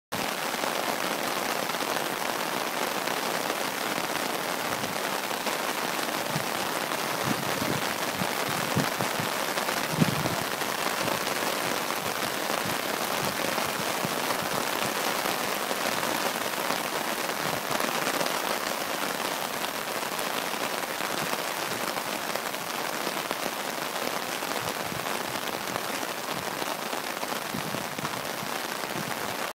Barclay's Scottish Open - 14th fairway being washed away
Torrential downpours on Saturday 8 July at Castle Stuart